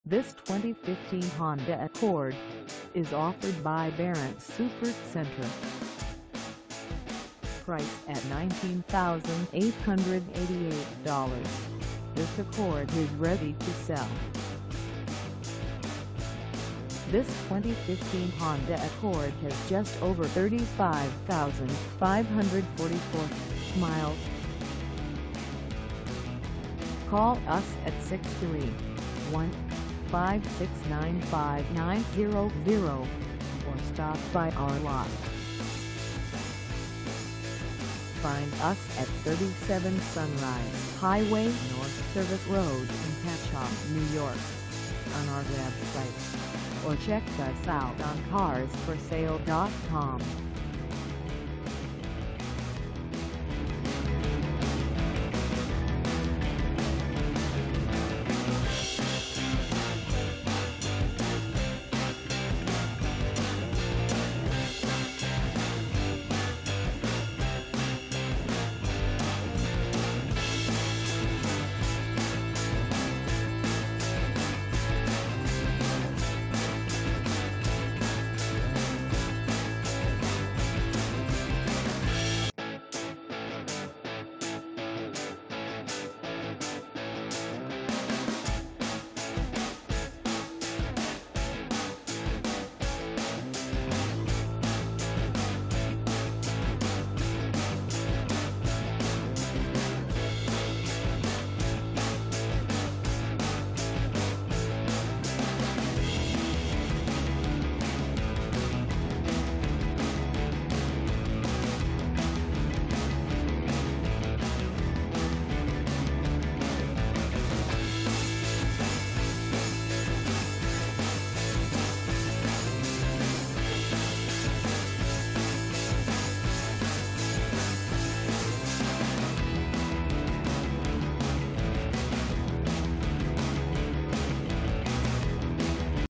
2015 Honda Accord EX-L for sale in Patchogue, NY 11772 at Ba
This 2015 Honda Accord EX-L is for sale in Patchogue, NY 11772 at Baron Super Center.

Engine: 3.5L V6
Transmission: Automatic 6-Speed
Color: White
Mileage: 35544
Contact Baron Super Center 37 Sunrise Hwy North Service Rd Patchogue, NY 11772 to test drive this 2015 Honda Accord EX-L today.